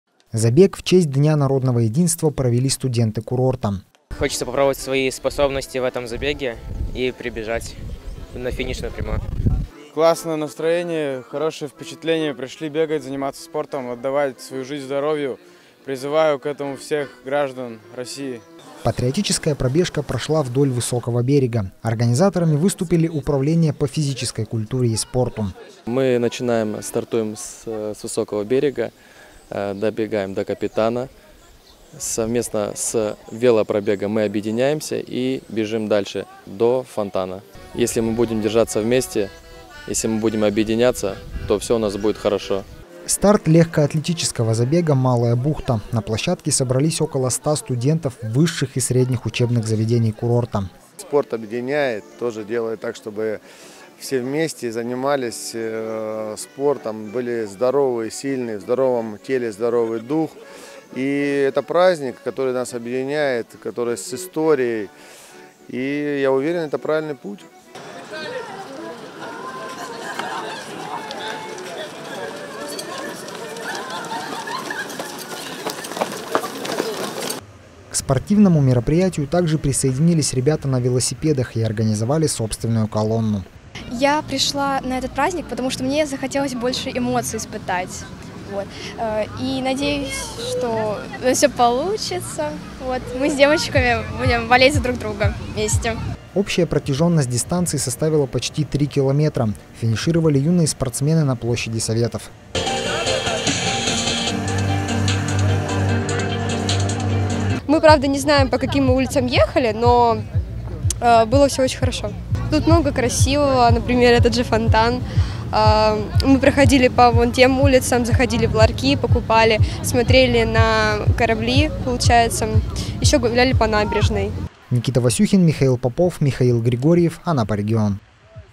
Забег в честь Дня народного единства провели студенты курорта
Патриотическая пробежка прошла вдоль Высокого берега. Организаторами выступили – управление по физической культуре и спорту.